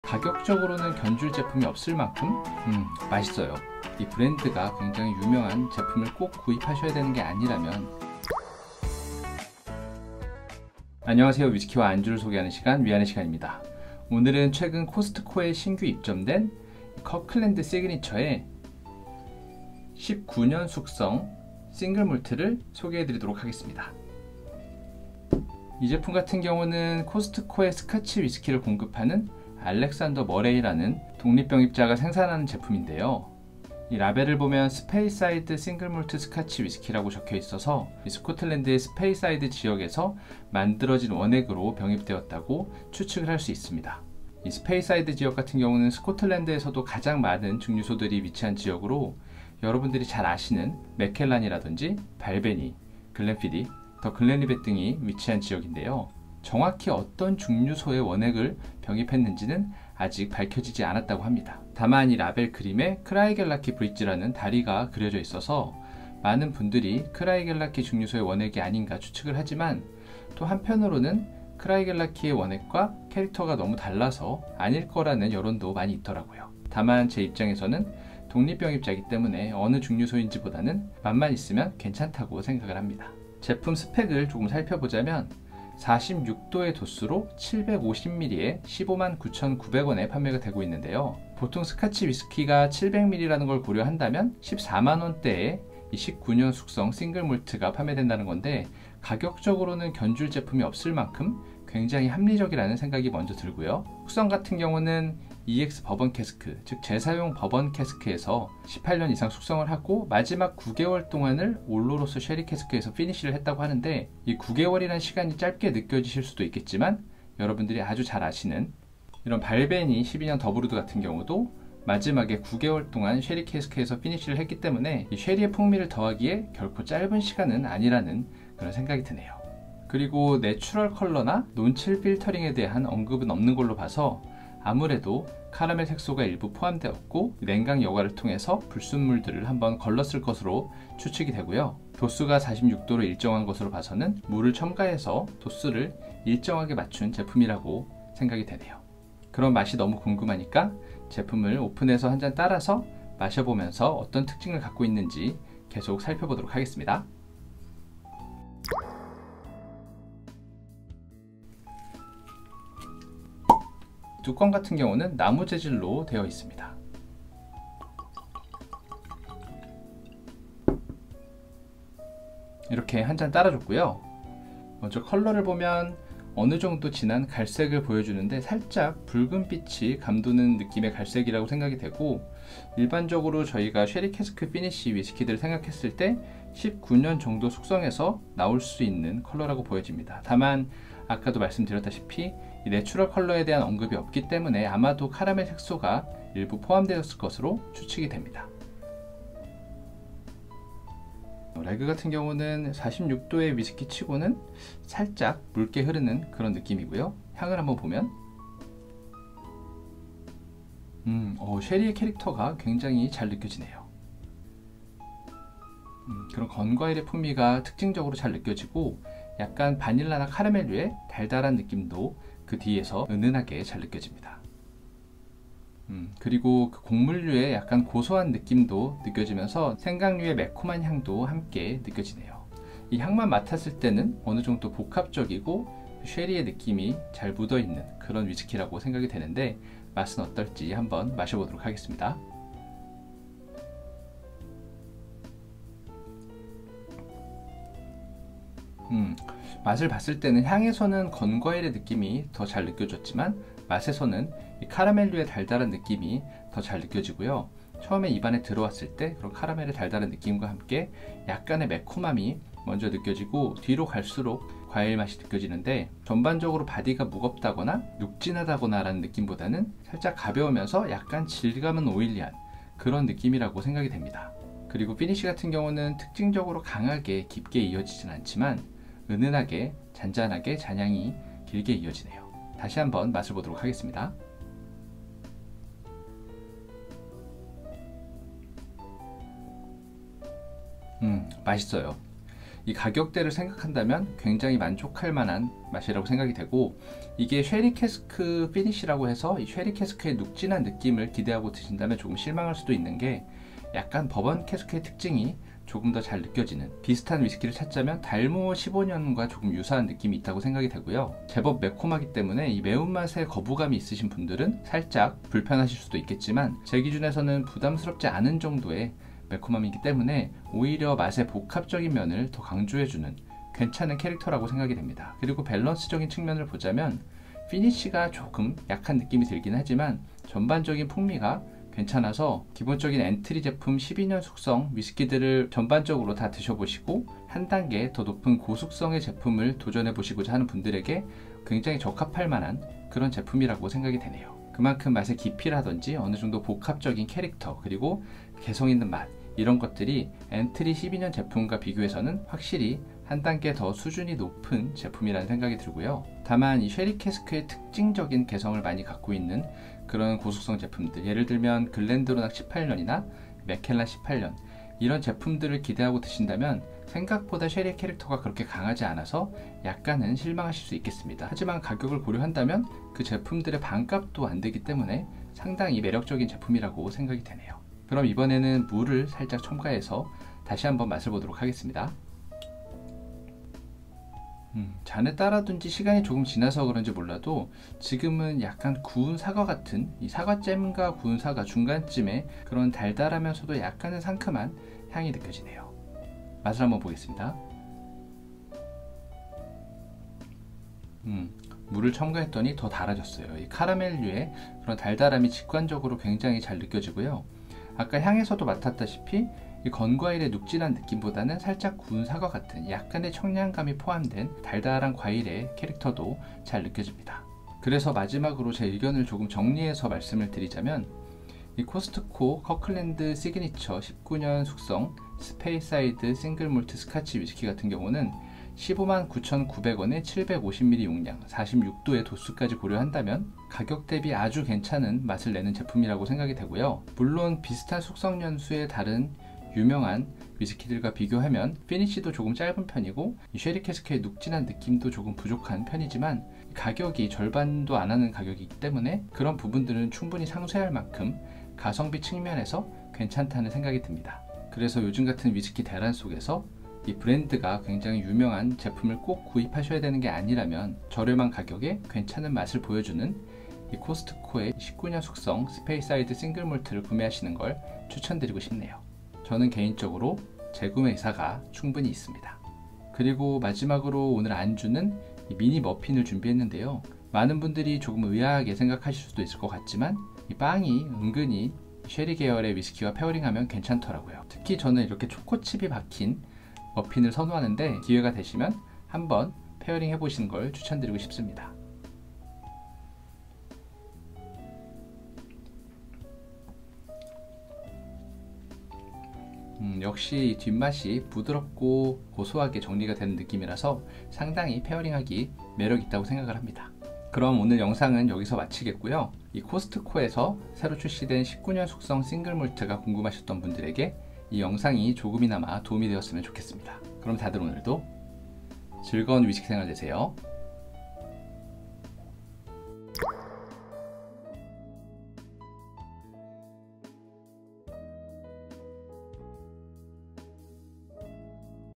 위스키 소개 #66- 코스트코 싱글몰트 19년 || 가격대비 맛이 훌륭한 셰리 피니시 위스키! 커클랜드 스페이사이드 싱글몰트 리뷰
코스트코에 신규 입점된 커클랜드 시그니처 스페이사이드 싱글몰트 스카치 위스키 19년 숙성 제품을 리뷰해 봤어요~!

750ml 용량에 159,900원인 이 위스키는 스페이사이드 지역의 증류소 원액으로 만들어졌는데요.

ex버번 캐스크 숙성 후 9개월간 올로로소 셰리 캐스크에서 피니시를 한 제품으로, 달콤한 카라멜 맛에 구운 사과 같은 과일의 캐릭터가 느껴져서 매력적인 제품이라고 느껴졌습니다.

이 제품이 궁금하셨던 분들에게 조금이나마 도움이 되었으면 좋겠네요^^!

그럼 다들 오늘도 즐거운 위스키 생활되세요🥃👍🏻